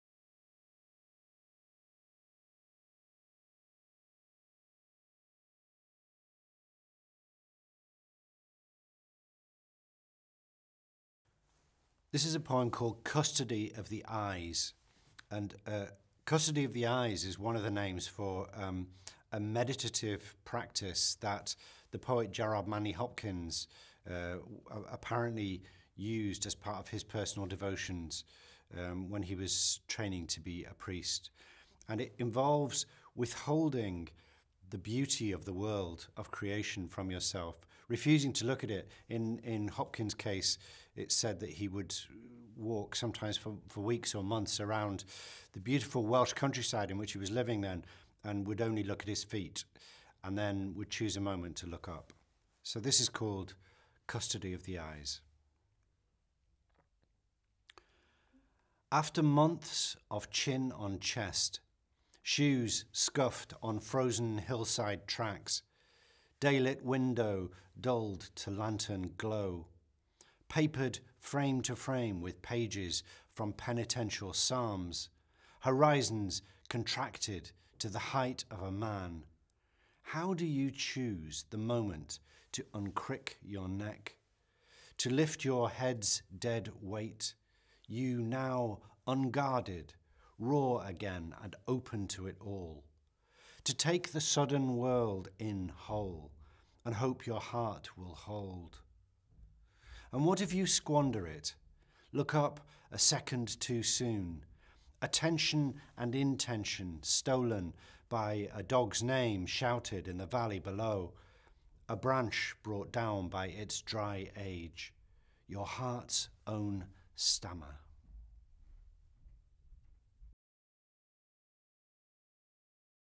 Michael Symmons Roberts reads 'Custody of the Eyes'
Michael reads his poem 'Custody of the Eyes' from 'Ransom', published by Cape Poetry and shortlisted for the T. S. Eliot Prize 2021.